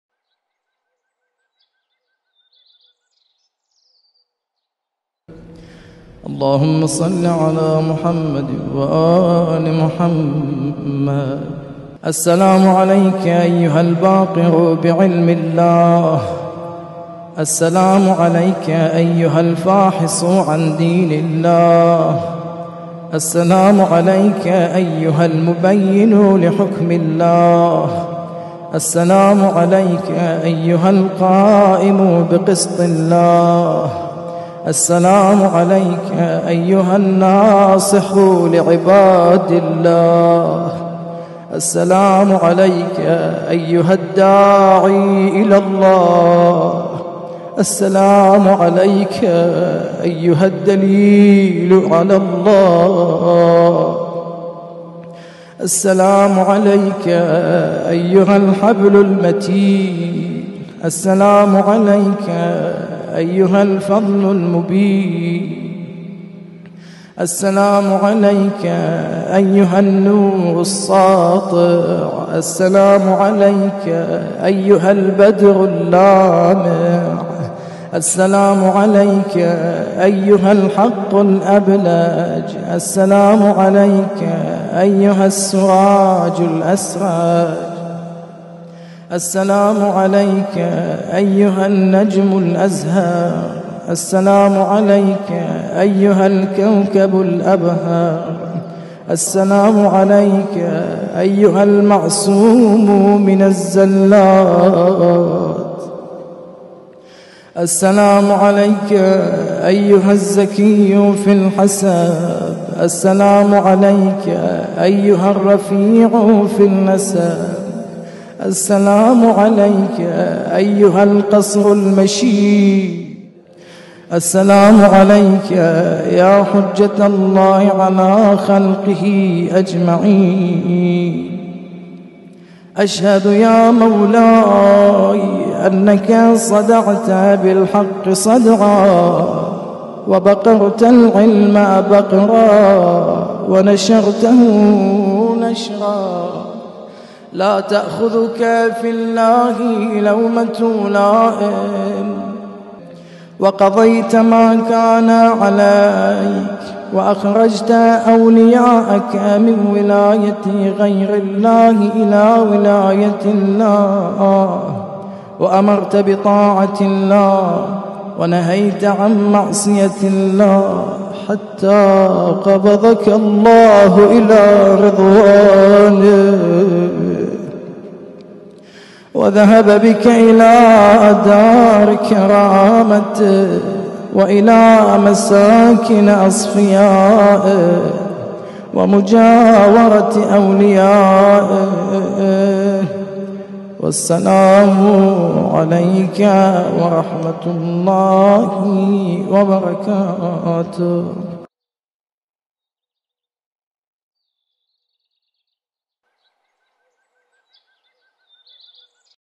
Ziyarat of Imam Muhammed Baqir (alaihis salaam)
Imam Mohammed al-Baqir presented the depth of various sciences to this world. So much so that it was the Messenger of Allah who gave him the title of Baqir—the one who splits open knowledge.

On his martyrdom, let's share our condolences to the Holy Prophet and his household. With the prayer that the shrines of Jannatul Baqi are rebuilt, and we can recite this Ziyarah standing next to the Imam's grave.